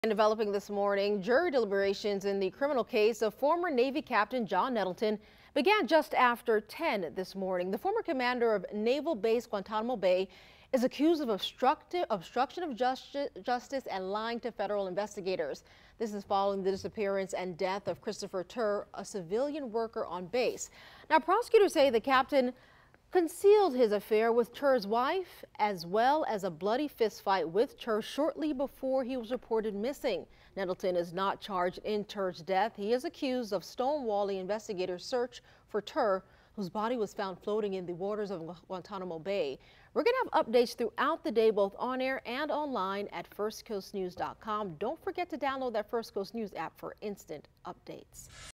Jury deliberations begin in trial of former Guantanamo Bay Navy Captain
He is accused of obstruction of justice and lying to federal investigators following the disappearance and death of Christopher Tur, a civilian worker on base.